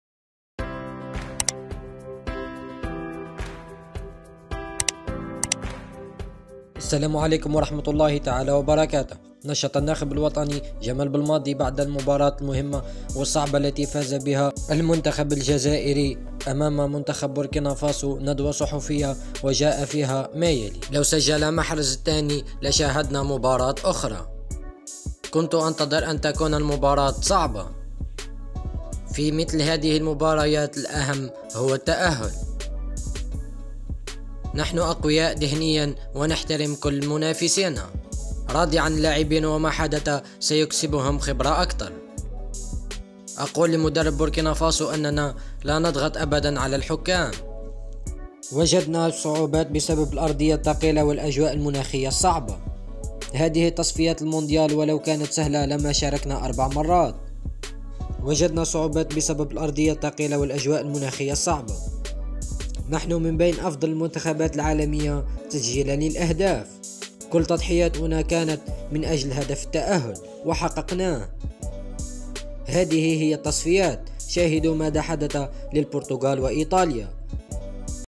تصريحات بلماضي بعد التأهل أمام بوركينا فاسو.2021.11.16
تصريحات بلماضي بعد التأهل أمام بوركينا فاسو حول ظروف المباراة و الصعوبات التي واجهها المنتخب الجزائري.